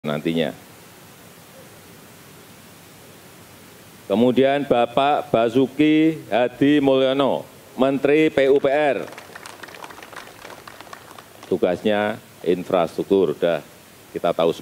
Jadi Menteri PUPR Lagi! Basuki Hadimuljono Lanjutkan Kelola Infrastruktur - Breaking iNews 23/10
Update informasi Anda bersama kami dalam Pengumuman dan Pelantikan Kabinet Jokowi-Amin - Breaking iNews Rabu, 23 Oktober 2019!